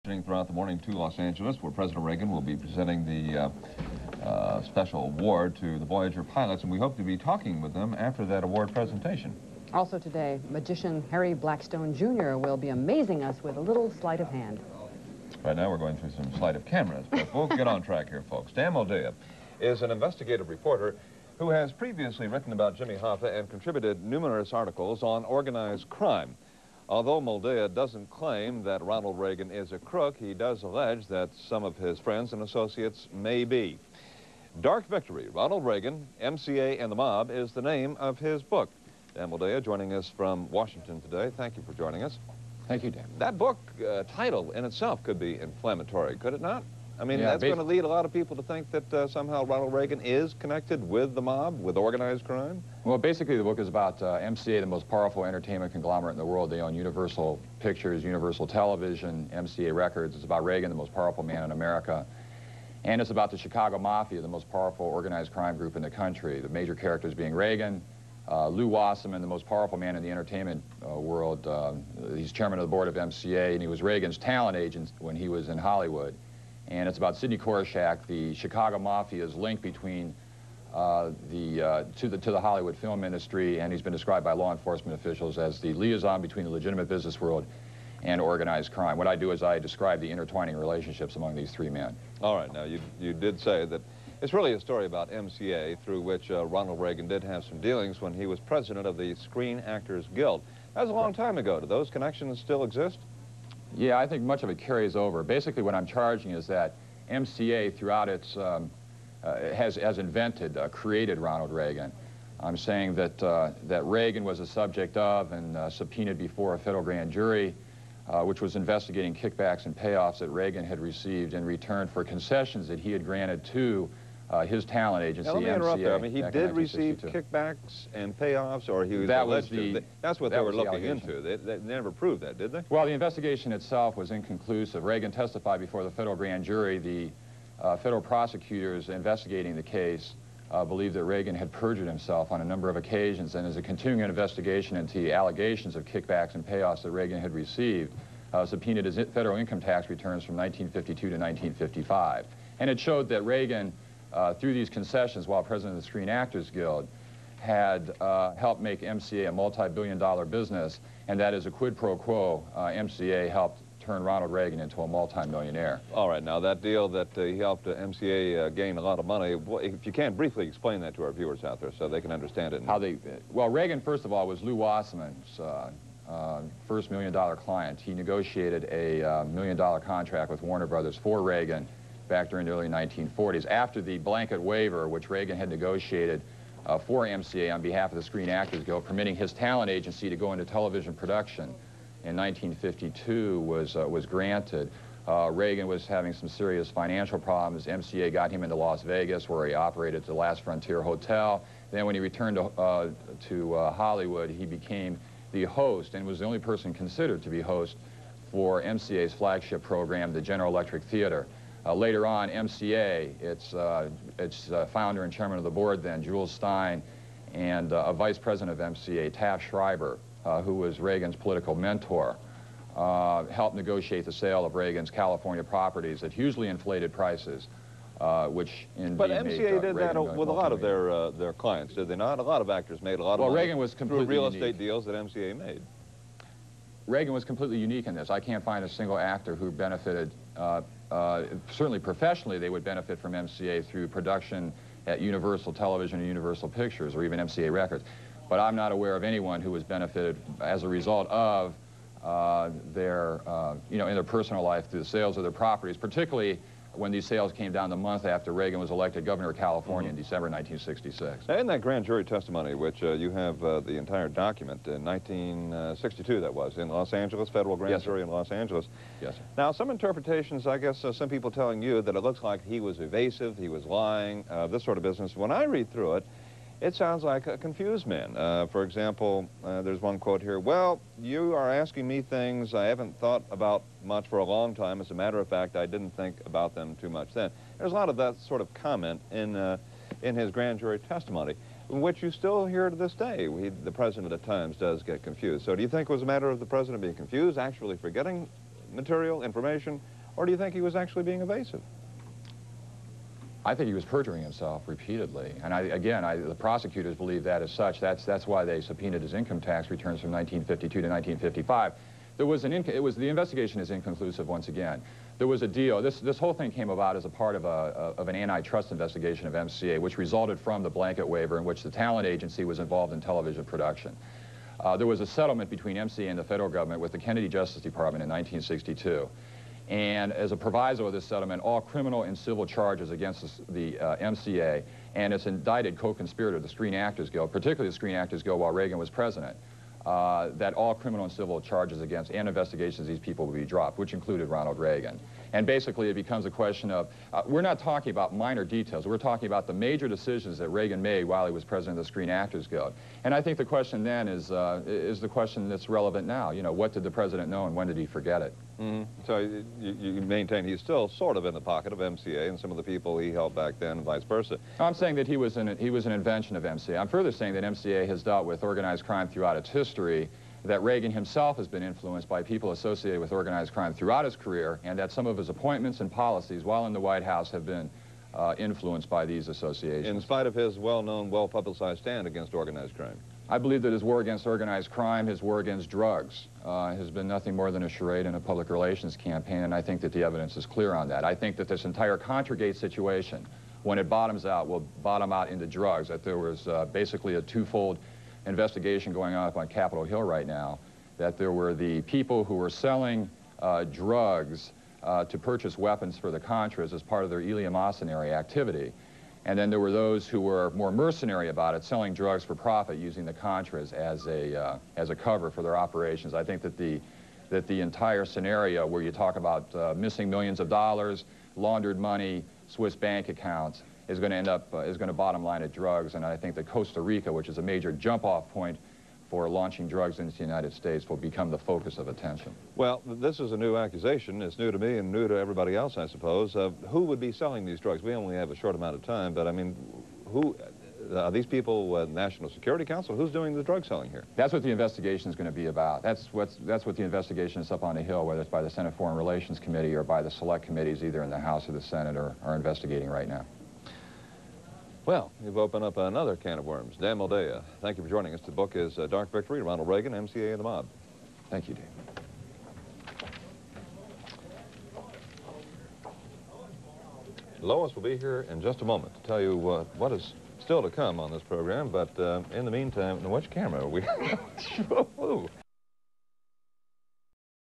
Moldea on MCA, the Mafia and the corruption of Ronald Reagan
Dan Moldea, author of the 1986 book, "Dark Victory:  Ronald Reagan, MCA and the Mob," explains the corruption of Ronald Reagan by MCA, his one-time talent agent--whom Reagan, while serving as president of the Screen Actors Guild helped become a multi-billion dollar business.  And then, as a quid pro quo, MCA helped Reagan become a multi-millionaire.  According to Moldea, MCA had ties to the Mafia throughout its history.